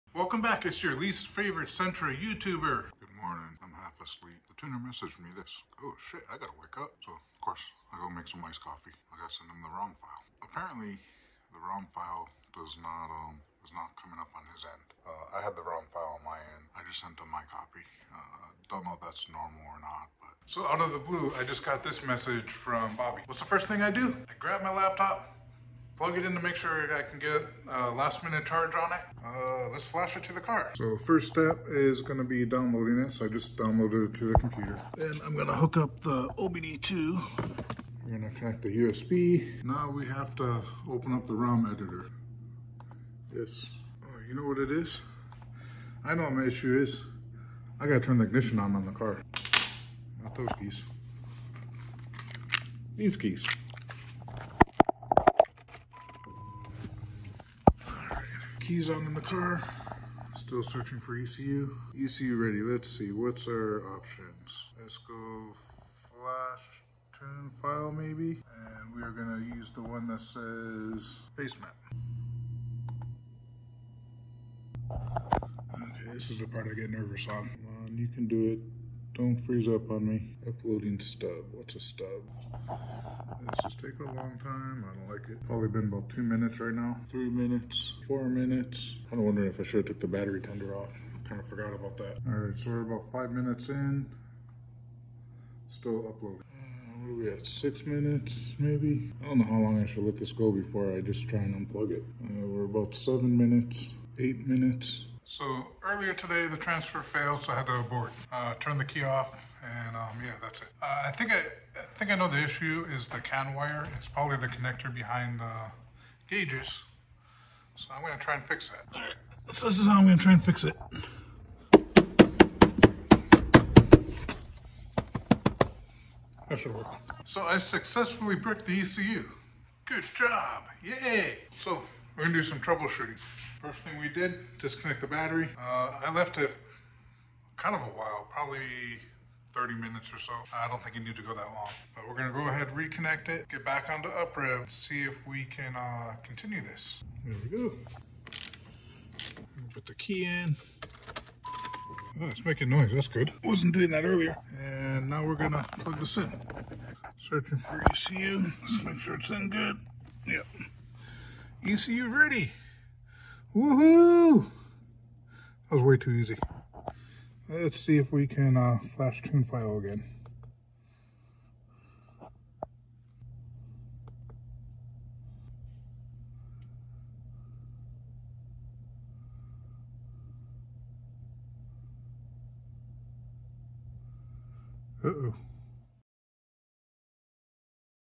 Did I Brick my sentra’s ECU? Part1 uprev on the b15 chassis
Turbo Manifold
TR8 intercooler
afr/boost guage
Turbo t31 flange
sr20 thermastat
injector adapter
intake filter
slim fans
oil pan
magnetic oil plug
inovate guage cord
rtv oil pan
fuel pump
coolent tank
universal pipe kit